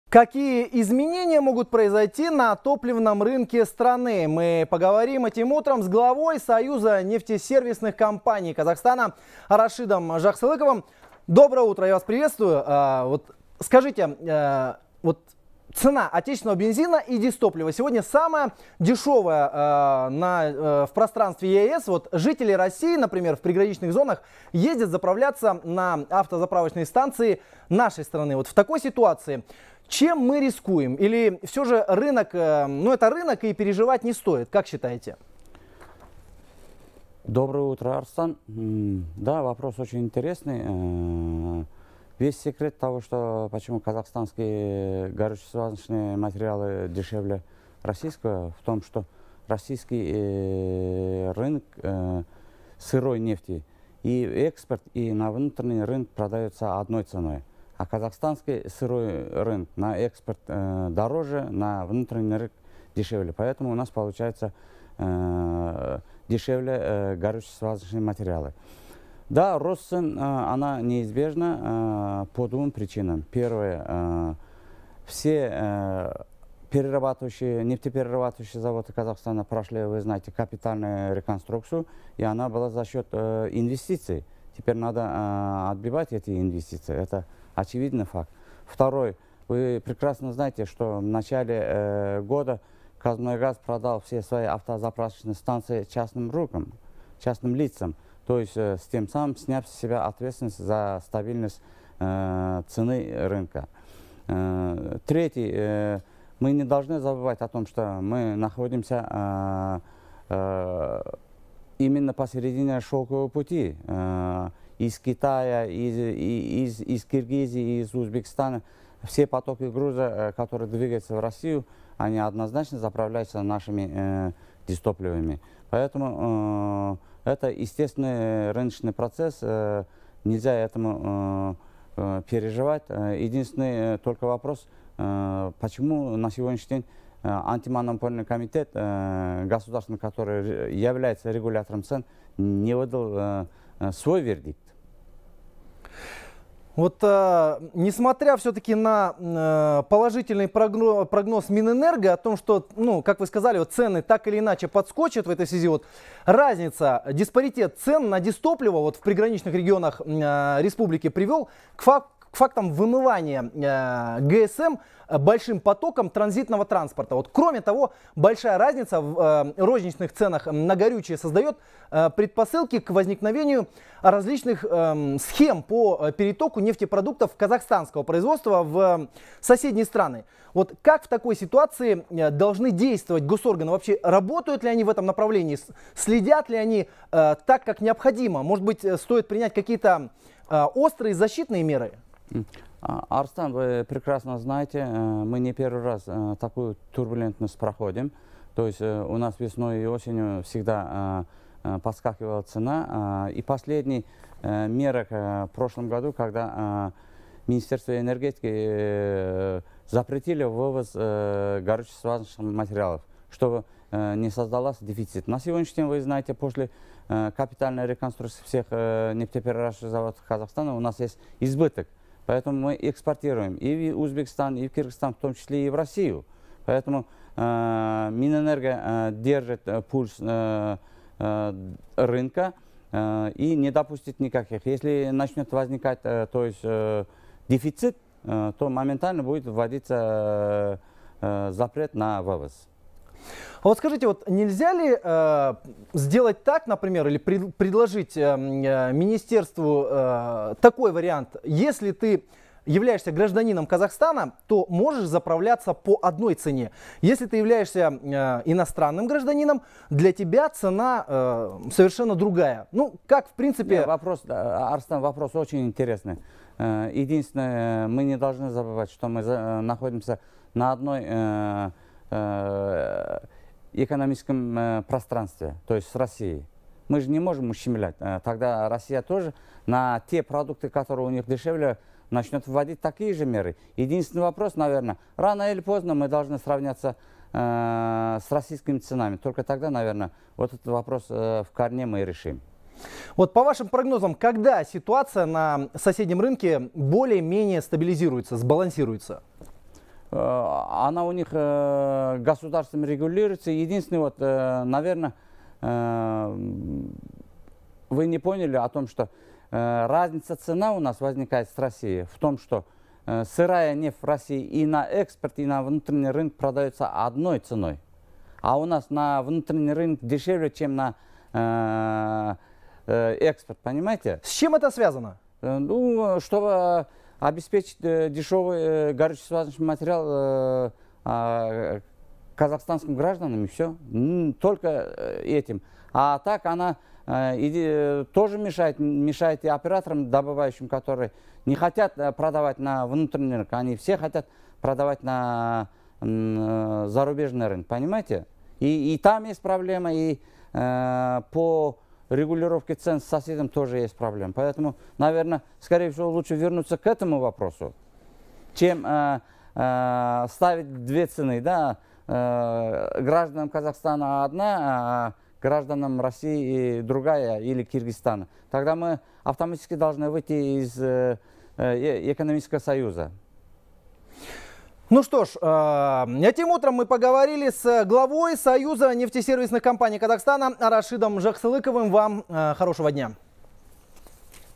Ситуация на топливном рынке Казахстана. Мнение эксперта
Рашид Жаксылыков - председатель президиума союза нефтесервисных компаний Казахстана;